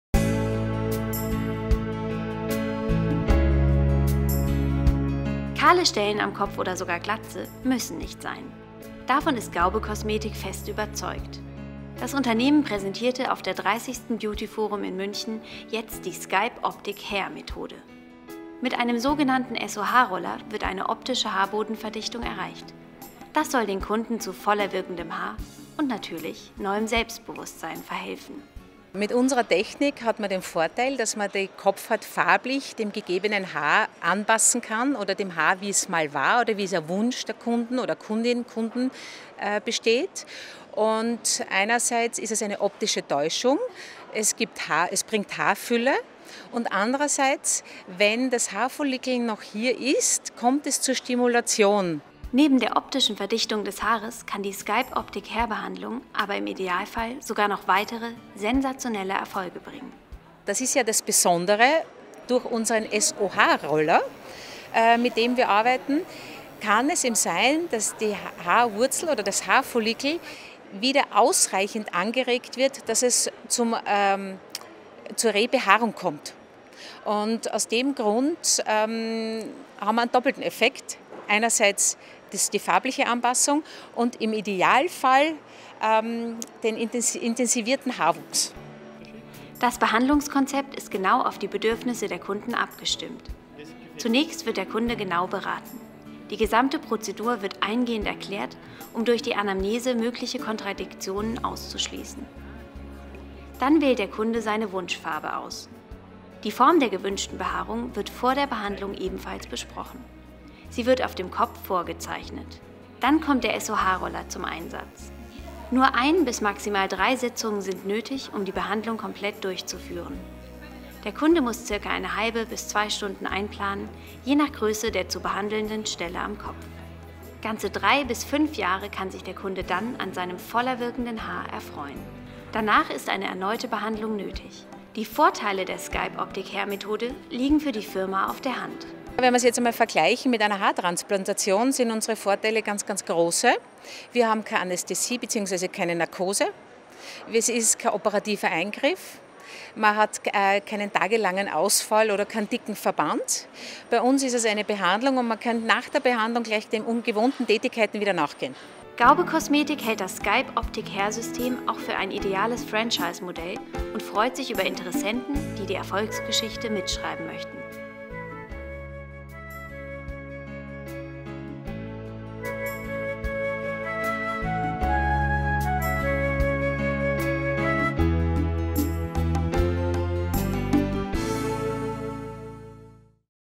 Gaube Kosmetik auf der BEAUTY FORUM MÜNCHEN 2015
MS Gaube Kosmetik zeigte auf der 30. BEAUTY FORUM München sein Rezept gegen Glatze und kahle Stellen am Kopf: den Scalp Otic Hair.